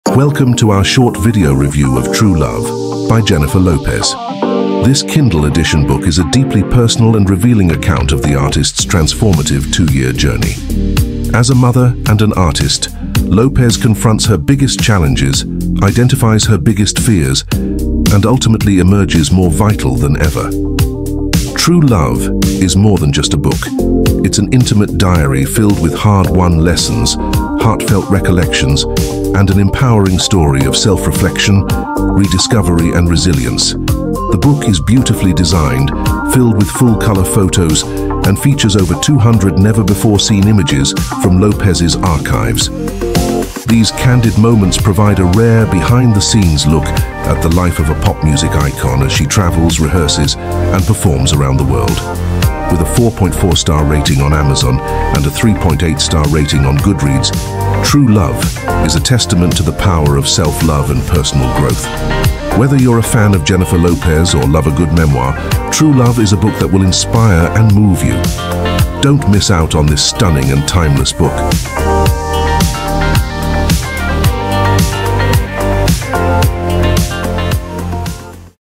“Jennifer Lopez: The True Love Journey That Shocked Fans Worldwide”
"Join us as we delve into the inspiring journey of Jennifer Lopez in her first-ever book, 'True Love.' This video explores JLo's transformative two-year journey, where she faced her most significant challenges, confronted her deepest fears, and emerged more vital than ever.

'True Love' is not just a book; it's an intimate diary filled with hard-won lessons, heartfelt recollections, and an empowering story of self-reflection, rediscovery, and resilience. The book features over 200 never-before-seen images from Lopez's personal archives, providing a rare behind-the-scenes look at the life of a pop music icon.

Whether you're a fan of Jennifer Lopez or love a good memoir, 'True Love' is a book that will inspire and move you. Please tune in to discover the power of self-love and personal growth, as revealed by JLo herself. 😊